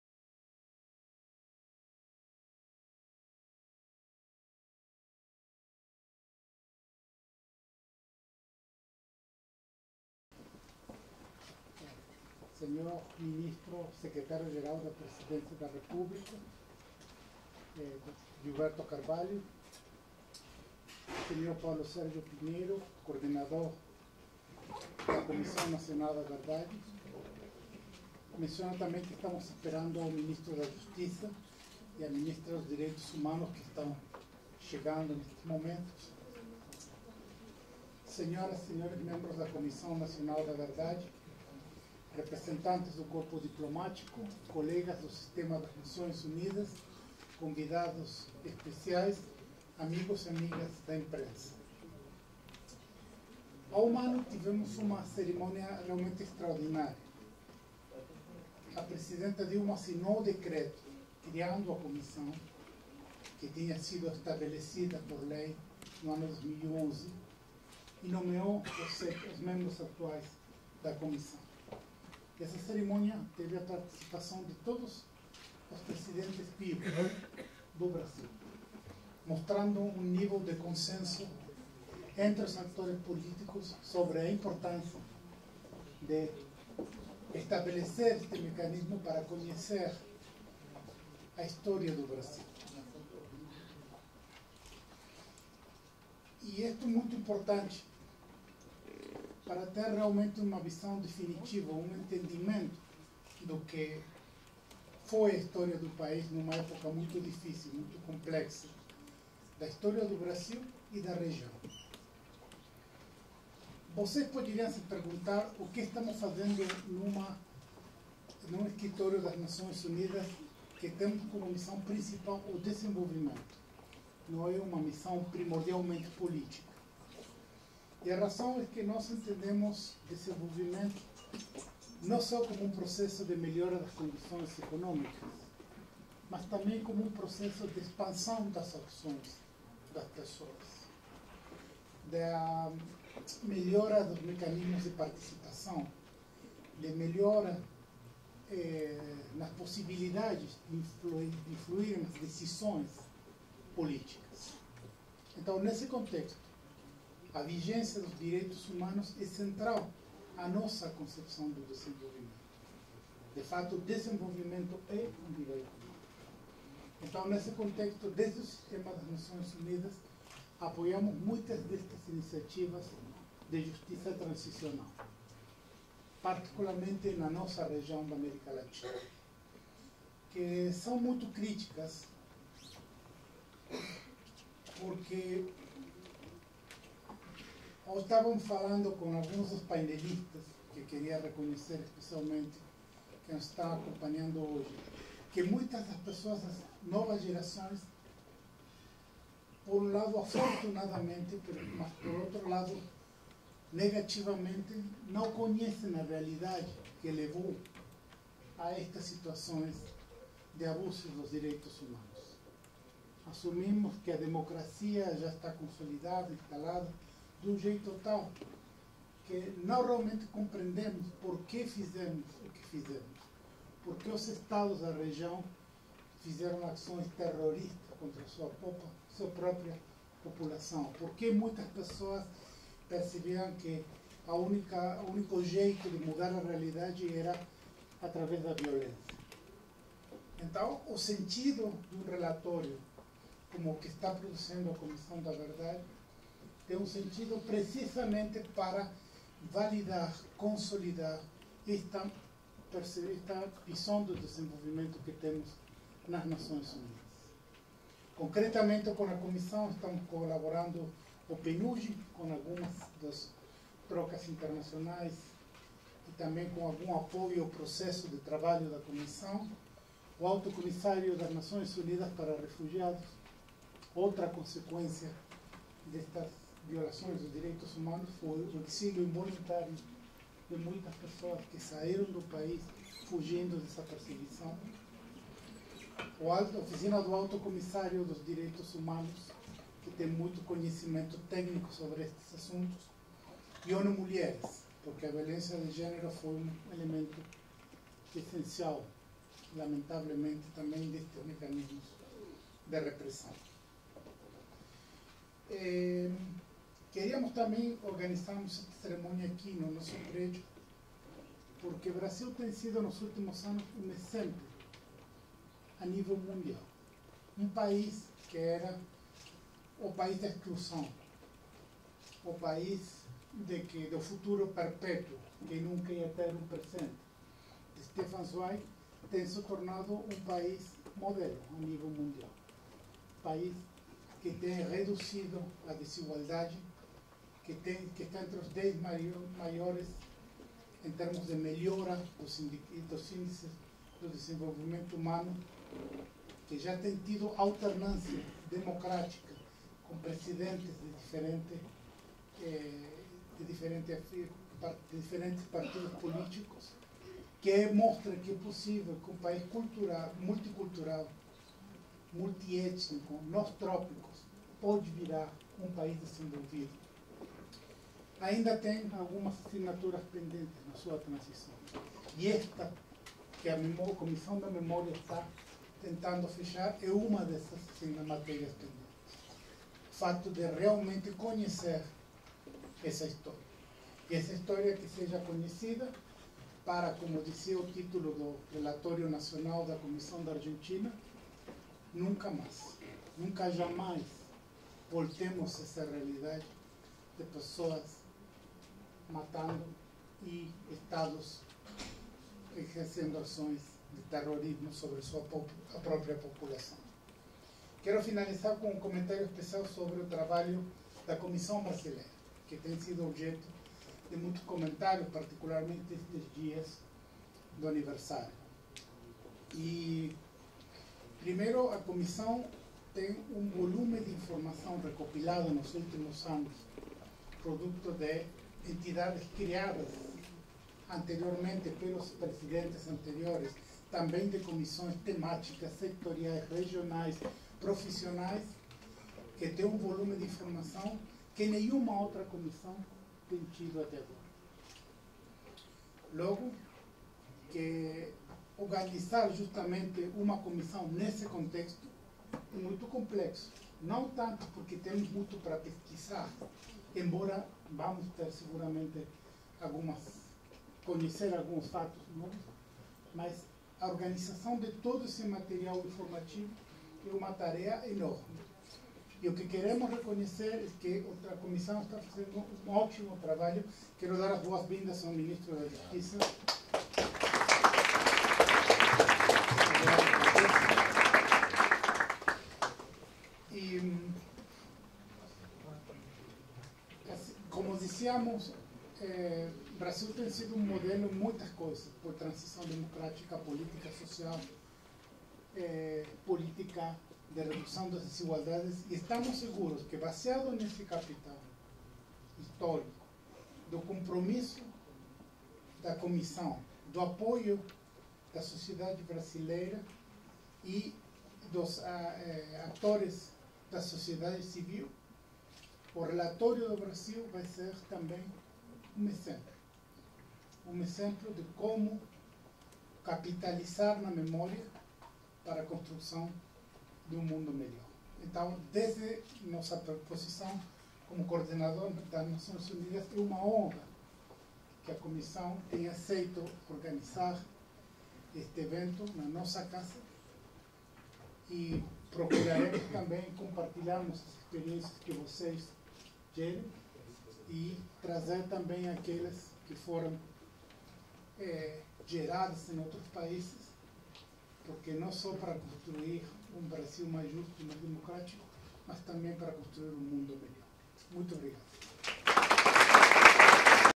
Seminário na ONU sobre 1 ano da CNV: Mesa de Abertura (1/2)
Jorge Chediek, coordenador do Sistema Nações Unidas no Brasil, fez a abertura do evento. "A Comissão Nacional da Verdade é muito importante para ter uma visão definitiva do que foi o país e a história do país e da região, que foi muito complexa", afirmou. Diferentes agências da ONU apoiam a CNV: PNUD, ACNUR e Onu Mulheres estão entre elas.

Data: 13/05/2013

Edição: Thiago Dutra Vilela (CNV)
Vinheta: Thiago Dutra Vilela (CNV)
Arte do canal: Paula Macedo e Isabela Miranda (CNV)
Captação de imagens e áudio: Empresa Brasil de Comunicação (EBC)